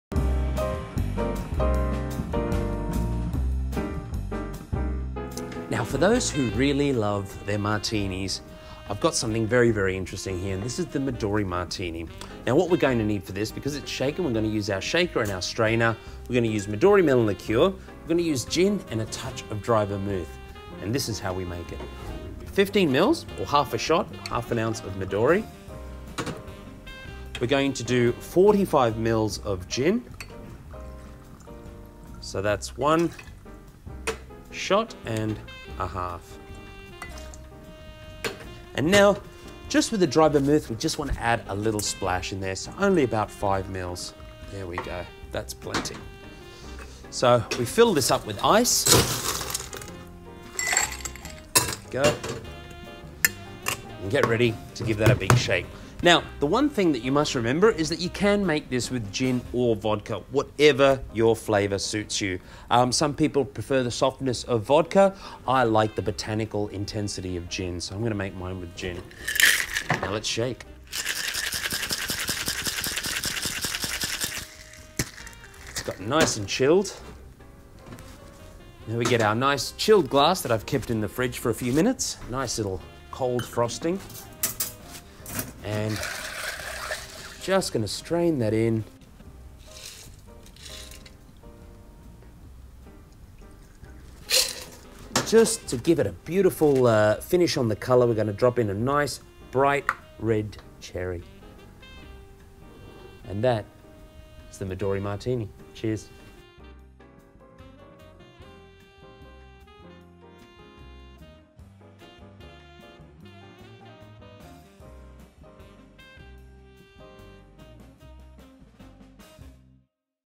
How to make a GIn based cocktail with MIDORI liqueur. "MIDORI Martini"
A recommendatory cocktail with MIDORI
 (ingredients) MIDORI、gin or vodka、Dash of dry vermouth
High alcoholic content
Dry flavor with Cocktail  glass
The martini  forever the King of Cocktails. Since its first appearance in the late 19th century, a number of variations on the classic martini have been developed, but these mostly consist of substituting the gin for another spirit, or adjusting the amount of vermouth. This changed at the end of the 20th century, however, when the cosmopolitan comeback sparked a whole new martini revolution. The variations developed during this craze were often served in glasses other than the traditional cocktail or martini glass. The MIDORI Martini is one of them. Enjoy its soft aromas as they envelop your mouth in a melon-flavored kiss.